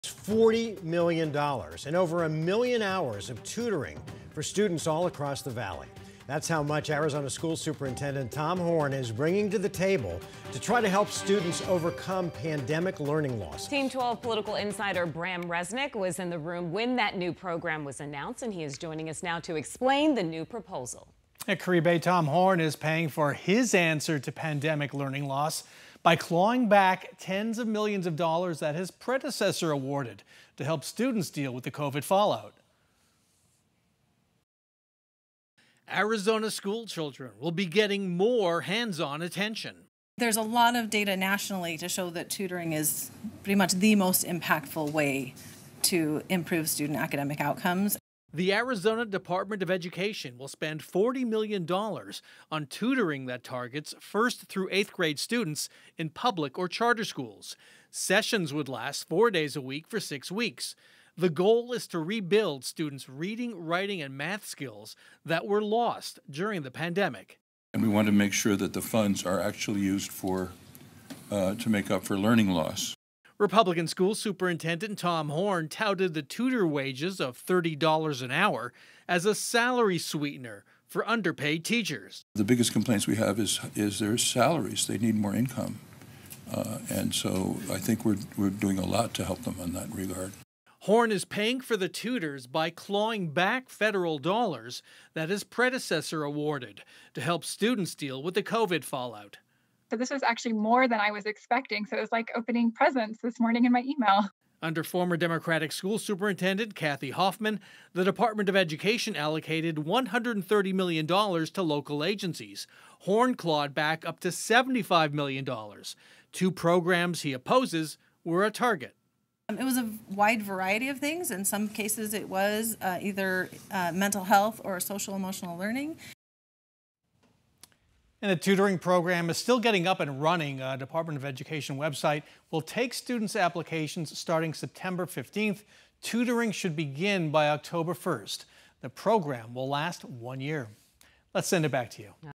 Tom Horne to use COVID relief for $40 million tutoring program
Only students between 1st and 8th grades at public and charter schools will be eligible. Parents can request it through the Arizona Department of Education website.